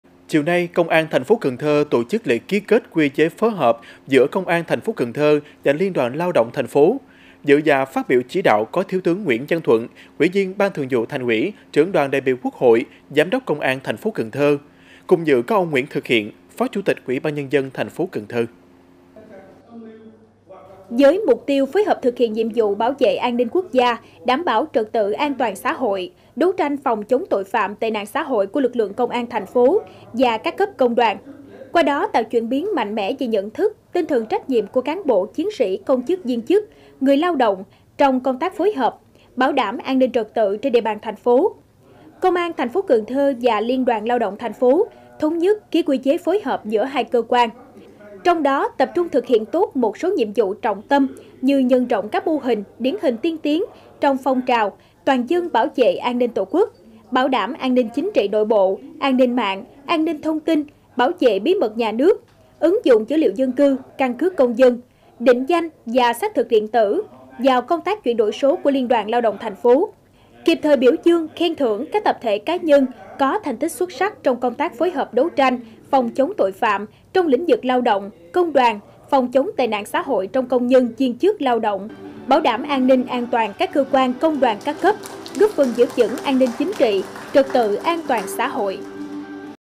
Ký kết quy chế phối hợp giữa Công an và Liên đoàn lao động TP. Cần Thơ| Cần Thơ TV
Chiều 4/3, Công an TP. Cần Thơ tổ chức lễ ký kết qui chế phối hợp giữa công an TP. Cần Thơ và Liên đoàn lao động thành phố.